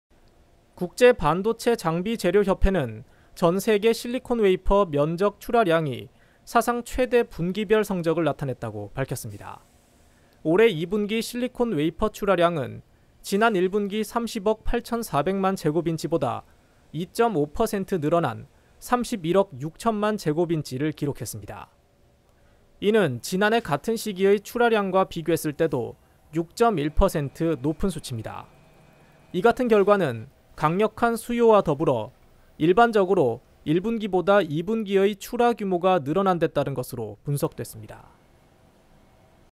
2분기 실리콘 웨이퍼 출하량 역대 최대_산업뉴스[산업방송 채널i] 
국제반도체장비재료협회는 전 세계 실리콘 웨이퍼 면적 출하량이 사상 최대 분기별 성적을 나타냈다고 밝혔습니다.

올해 2분기 실리콘 웨이퍼 출하량은 지난 1분기 30억 8,400만 제곱인치보다 2.5% 늘어난 31억 6,000만 제곱인치를 기록했습니다.

이는 지난해 같은 시기의 출하량과 비교했을 때도 6.1% 높은 수치입니다.

이 같은 결과는 강력한 수요와 더불어 일반적으로 1분기보다 2분기의 출하 규모가 늘어나는 데 따른 것으로 분석됐습니다.